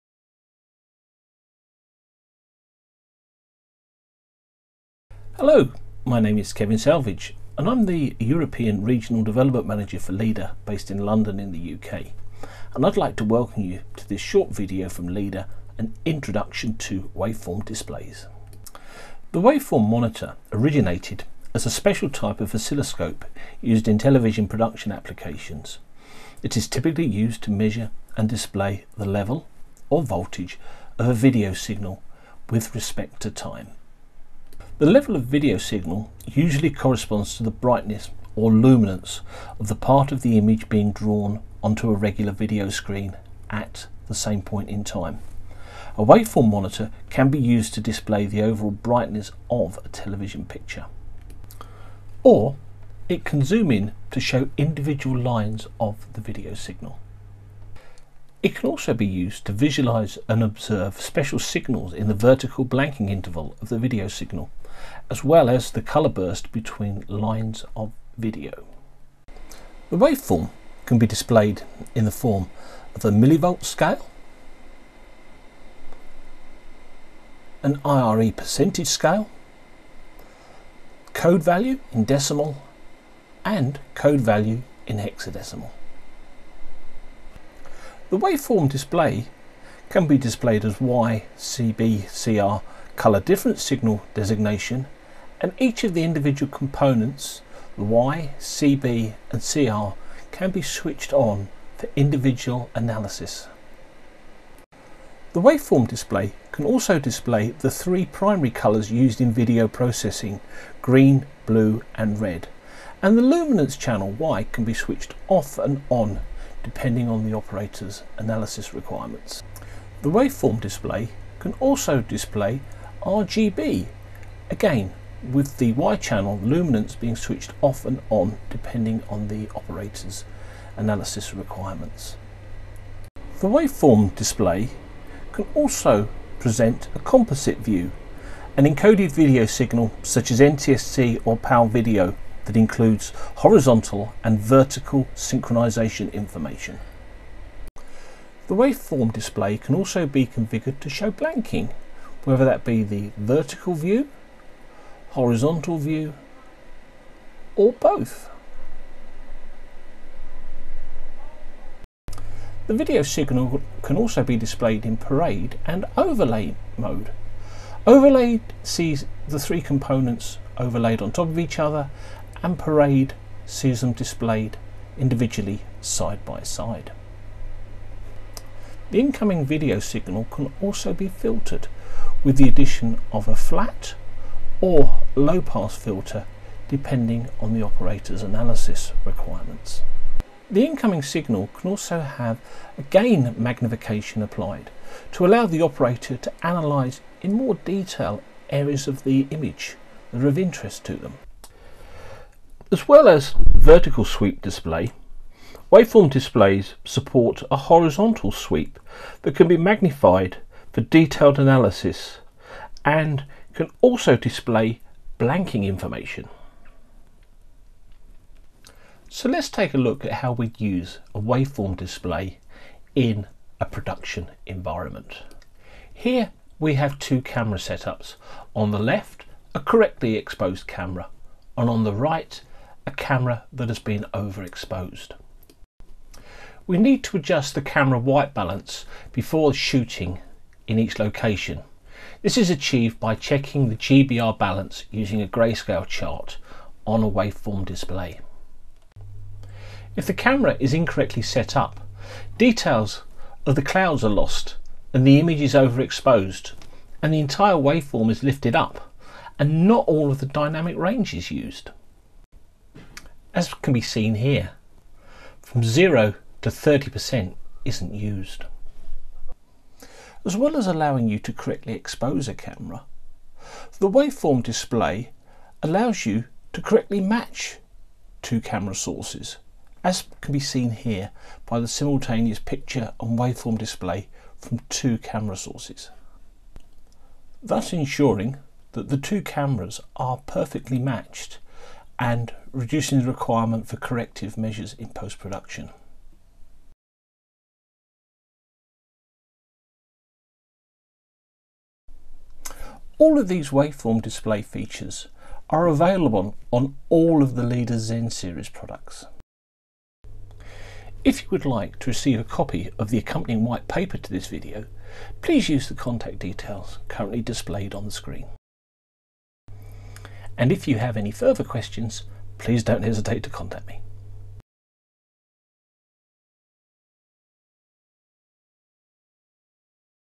Leader An Introduction to a Waveform Display
The Leader ‘An Introduction to ...’ is part of a series of tutorials in which the fundamental principles of video and audio test and measurement will be explained.

This tutorial focuses on the waveform display, its origins and how it is used in film and television production.